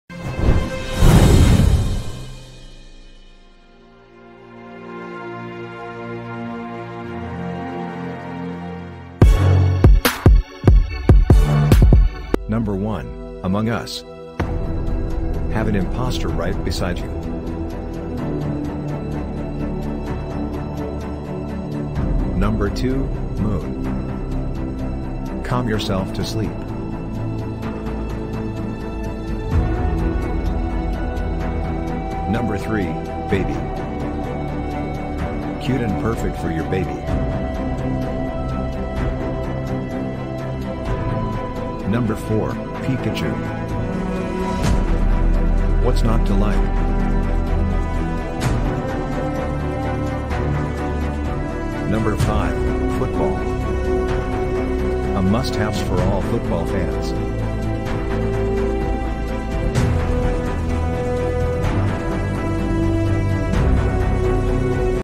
5 Bedside Night Lights You Need From Shopee For Under $10 | Shopee Must Haves
Check out these trendy bedside lights that will brighten up your night, every night this July 2021!